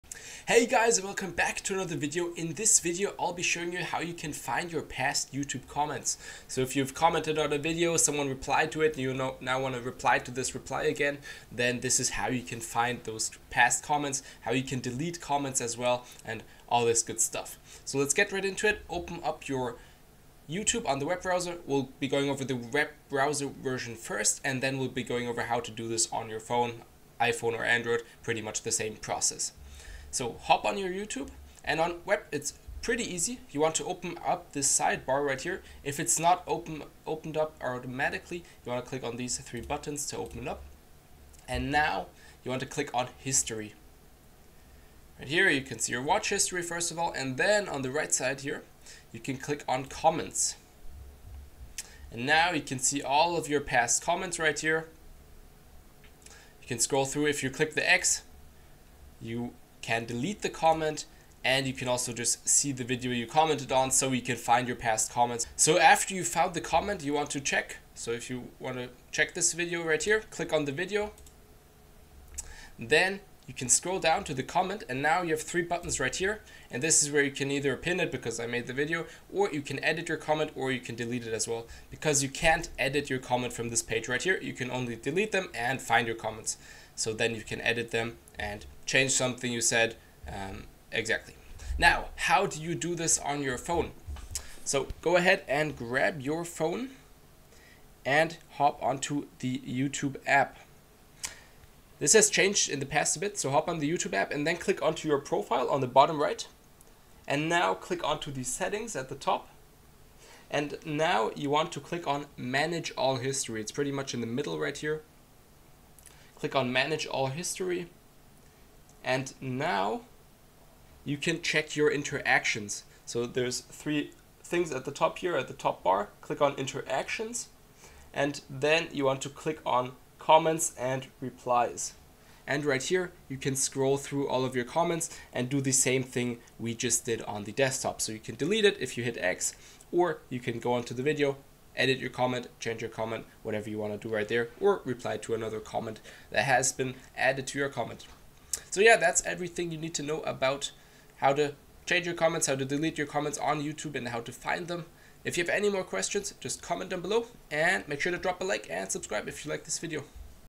How to Find Your Comments on Youtube (Mobile & Desktop)
Commenting on videos is a fun way to engage with others, but tracking your comment history can be tricky. In this video, we’ll show you how to find your past YouTube comments, reply to replies, and delete comments if needed. Let’s dive in!

⏰ Chapters:
0:00 - Introduction
0:18 - Finding Your YouTube Comments (Desktop)
0:55 - Managing Your YouTube Comments (Desktop)
1:32 - Finding & Managing Your YouTube Comments (Mobile)

📋 Key Steps:

💻 Finding Comments (Desktop):
➡️ Open YouTube in your web browser.
➡️ Click on "≡" → "History" → "Comments."

💻 Managing Comments (Desktop):
➡️ To delete a comment, click the "X" next to it.
➡️ Click on the video to navigate to your comment and manage it (edit, delete, or pin).

📱 Finding & Managing Comments (Mobile):
➡️ Open the YouTube app.
➡️ Tap on your profile → "Settings" → "Manage All History."
➡️ Select "Interactions" → "Comments and Replies."
➡️ You can delete comments or go to the video to edit or reply.